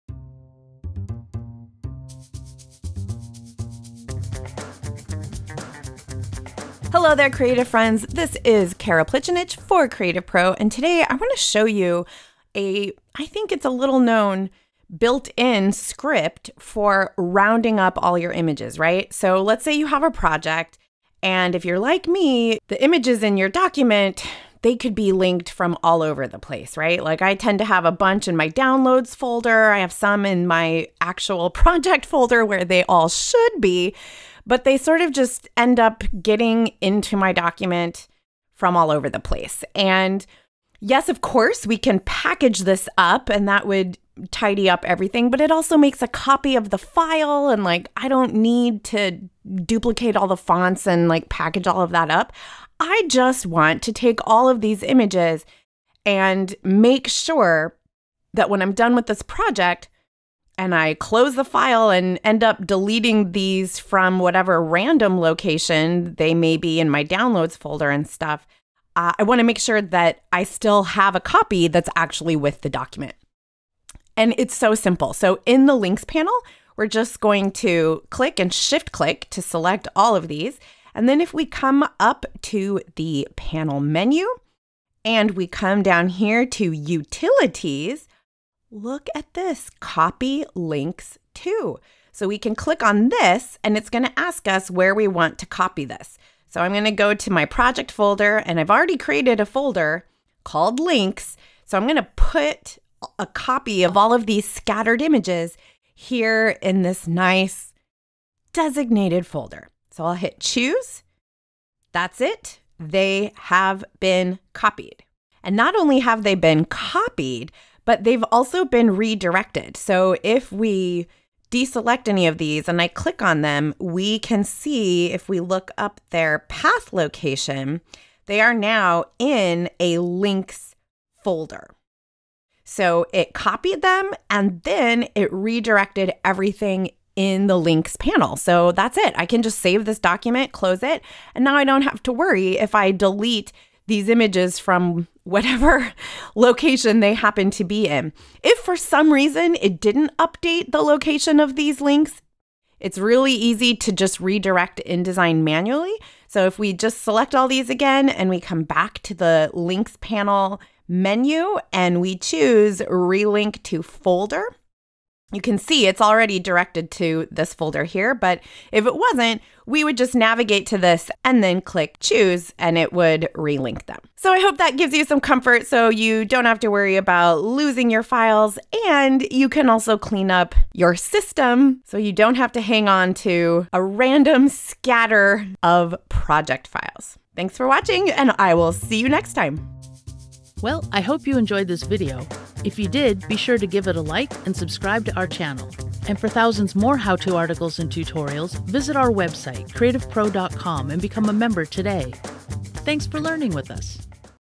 InDesign: How to Collect All Links in a File (Video Tutorial)
In this InDesign how-to video, Khara Plicanic shows how easy it is to gather up all the links used in a file and put them in one location. If you’ve placed graphics and photos into your InDesign layout from all over your computer, at some point, you’ll probably want them all in one place. Khara demos how to do that with a couple of clicks.